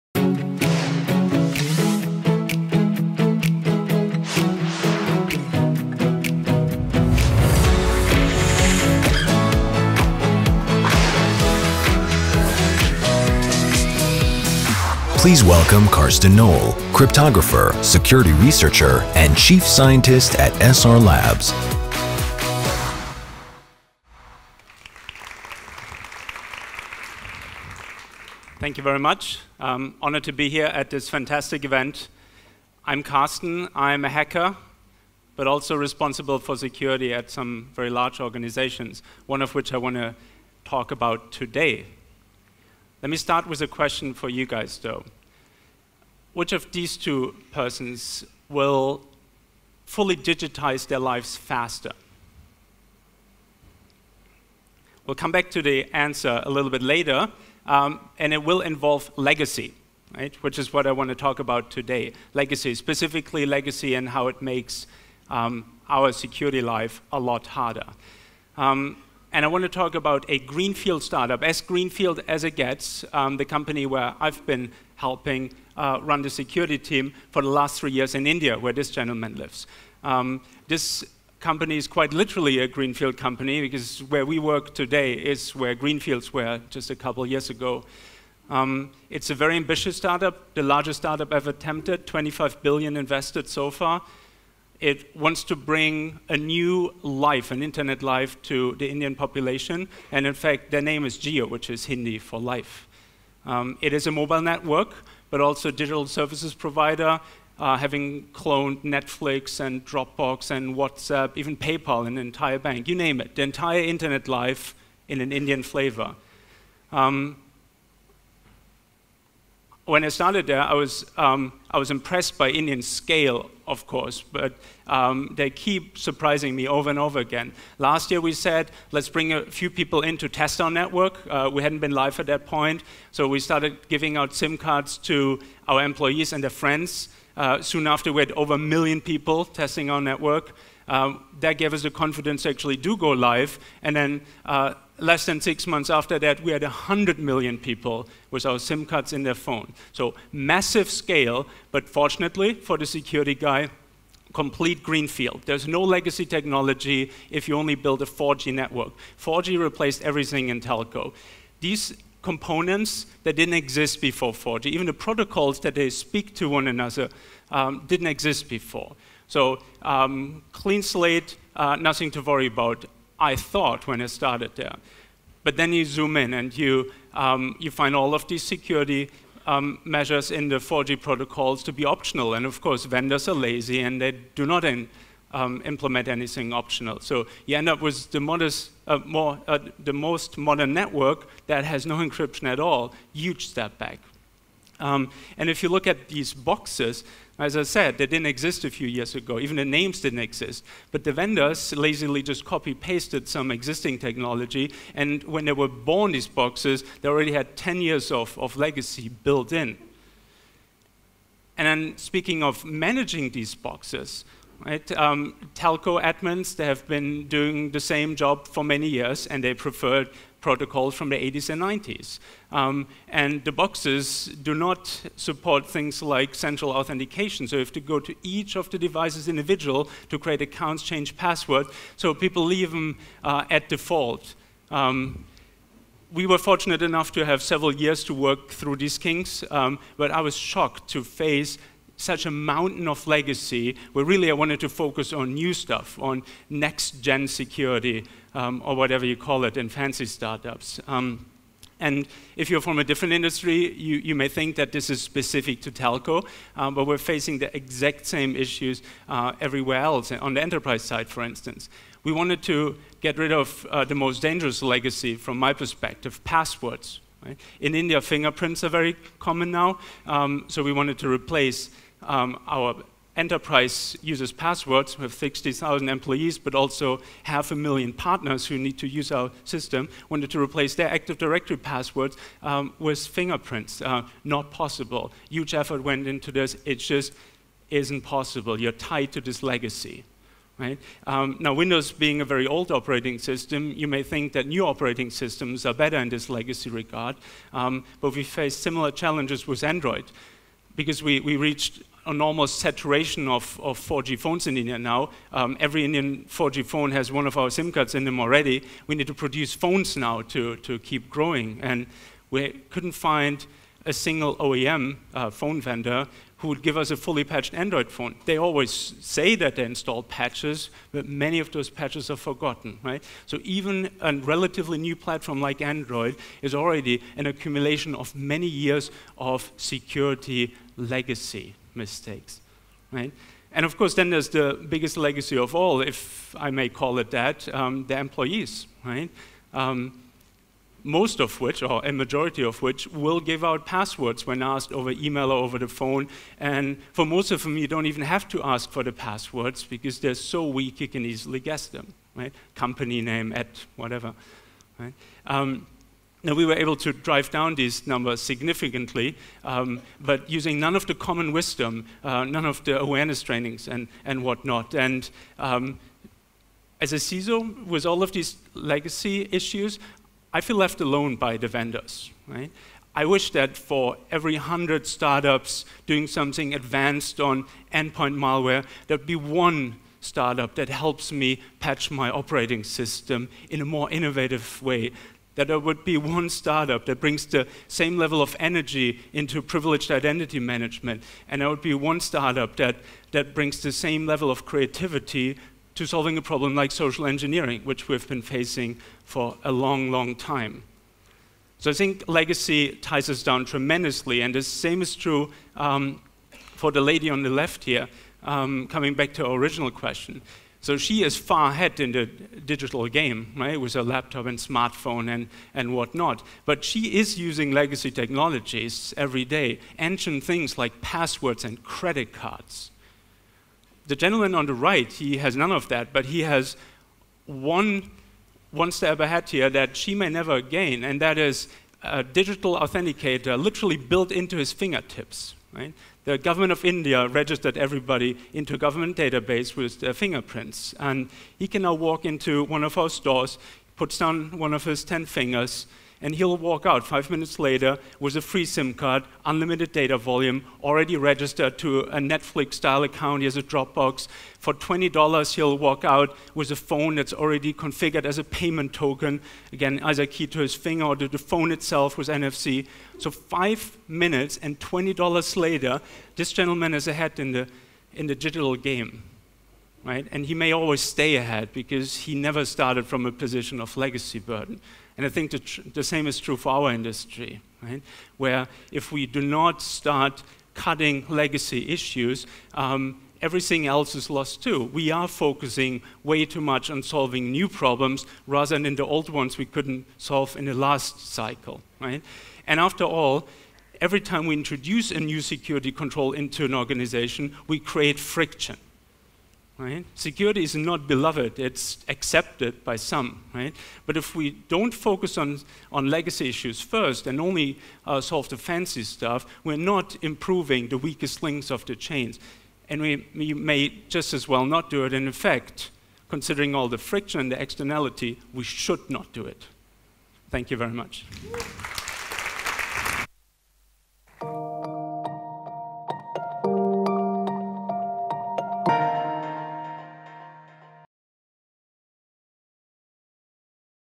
Rethink Cyber TLV 2017 - Karsten Nohl, Chief Scientist at SRLabs
Karsten Nohl, Chief Scientist at SRLabs, discusses "Uprooting Legacy Weeds in Technology Greenfields" at Rethink Cyber Tel Aviv 2017